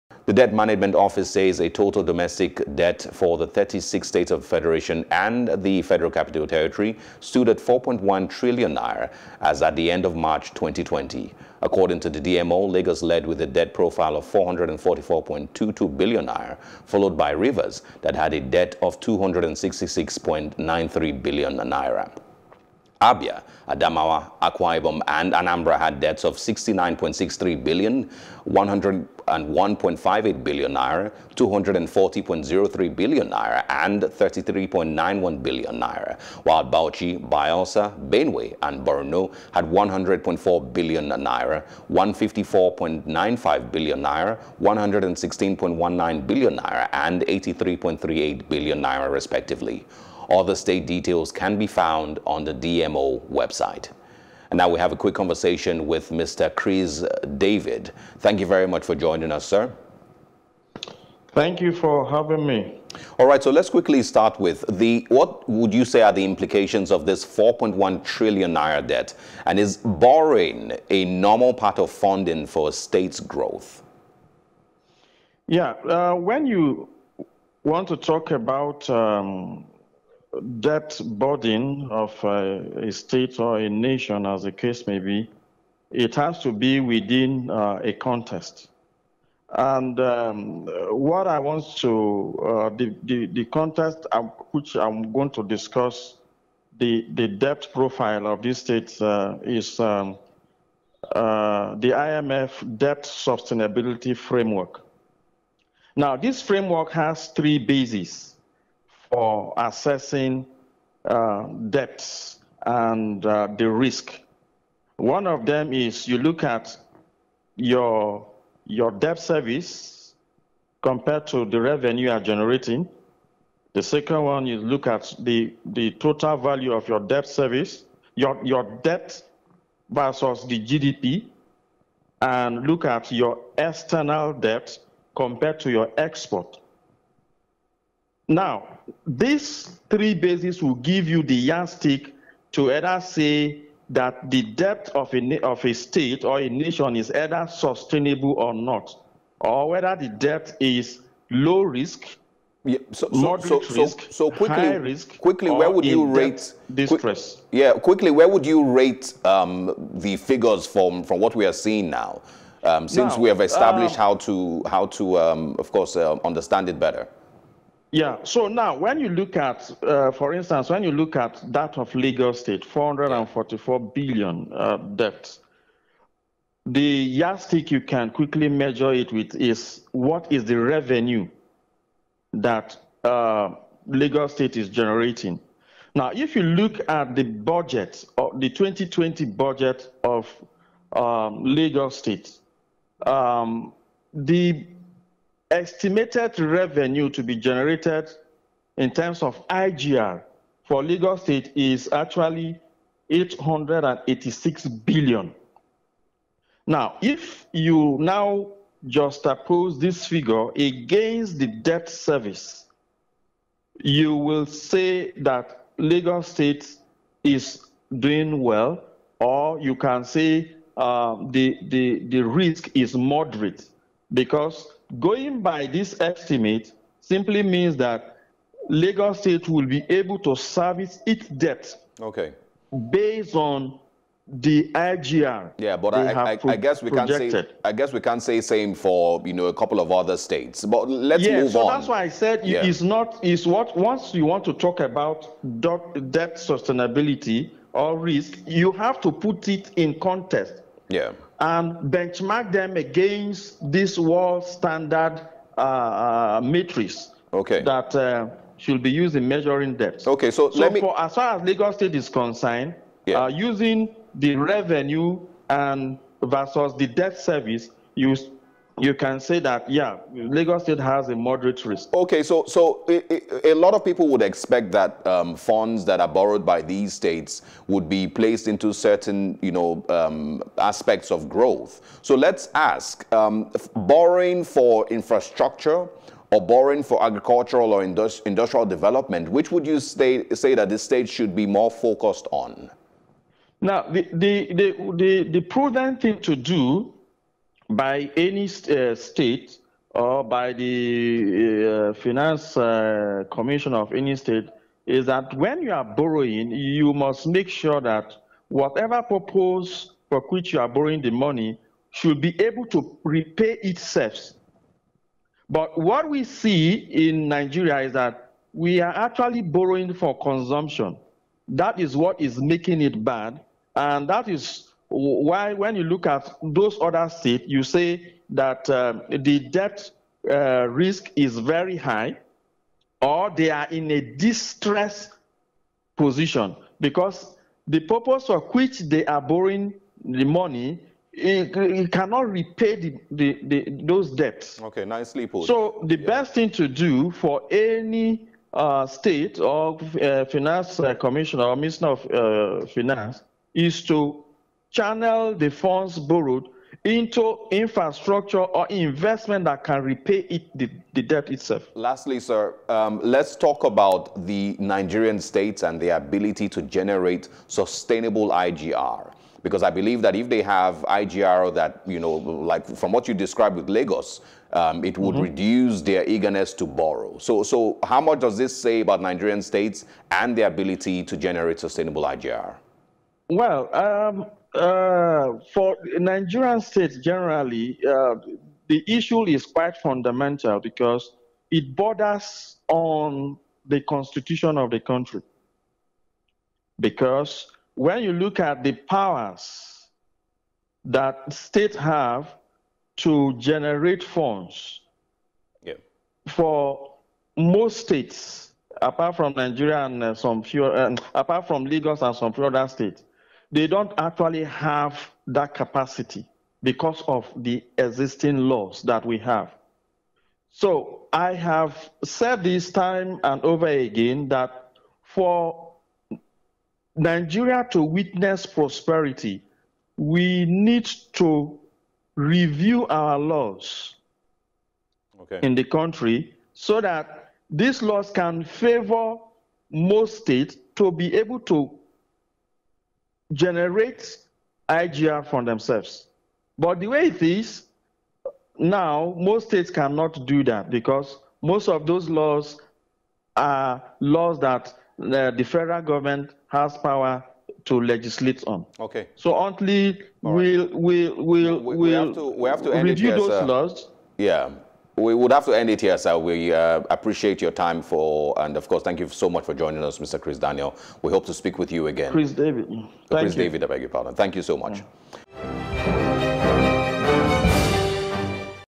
Nigeria Borrow Loans For Consumption Not Investment - Kriz David
The Debt Management Office says the total domestic debts for the 36 states of federation and the Federal Capital Territory stood at N4.1tn as of the end of March, 2020.

Kriz David, CEO, KrizDavid Foresigh joined Plus TV Africa on the news.